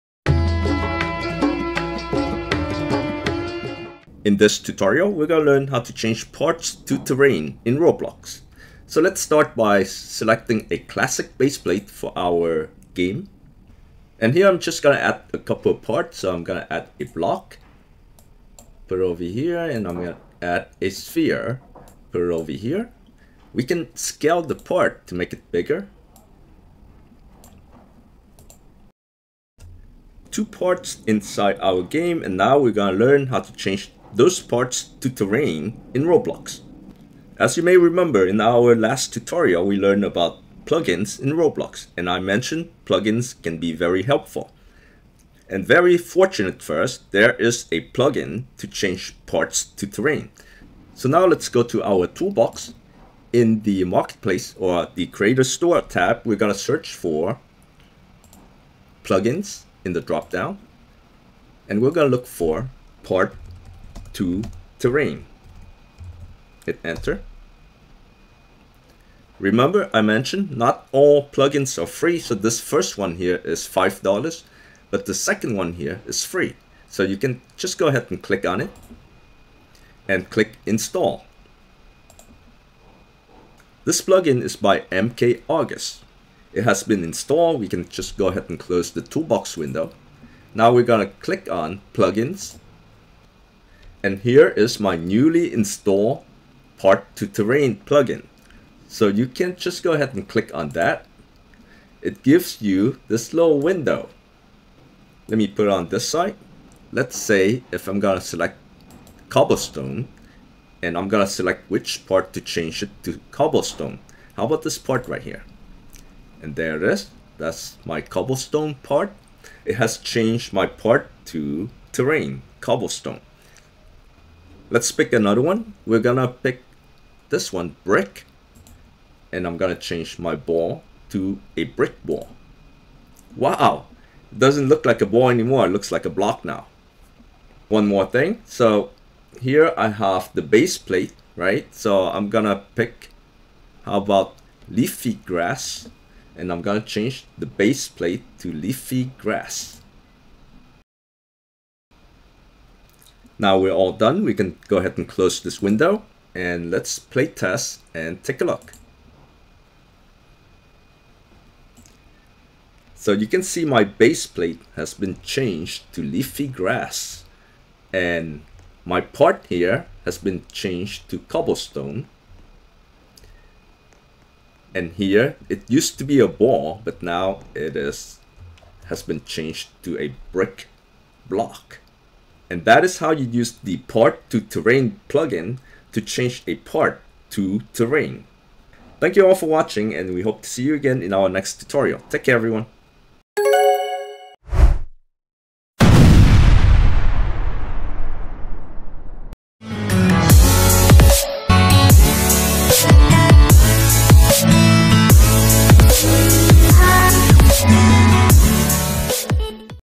How to change a part to terrain in Roblox
In this Roblox Studio tutorial, you will learn of an easy way to change a part to terrain in Roblox.